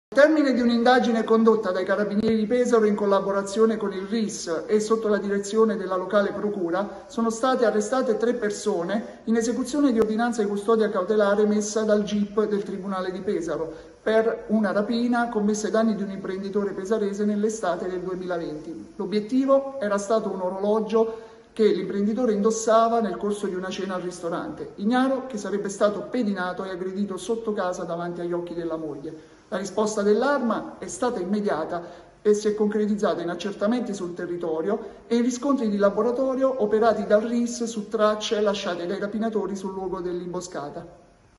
Imprenditore rapinato dell'orologio da 40mila euro: 3 arresti
intervento del Maggiore Spina